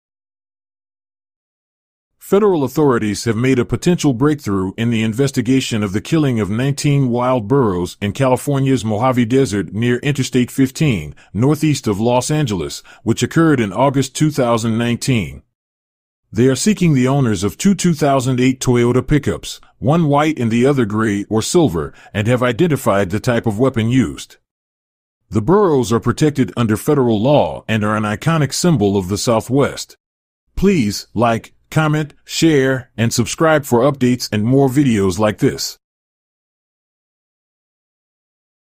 Investigation of 19 Wild Burros Killed in Mojave Desert
Federal authorities have made a potential breakthrough in the investigation of the killing of 19 wild burros in California's Mojave Desert near Interstate 15, northeast of Los Angeles, which occurred in August 2019. They are seeking the owners of two 2008 Toyota pickups, one white and the other gray or silver, and have identified the type of weapon used. The burros are protected under federal law and are an iconic symbol of the Southwest​​.